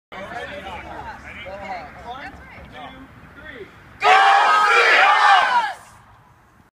Measuring Sparling's Loudest 12th Man Cheer
Cheering on our Seahawks we measured 120dB on the sound level.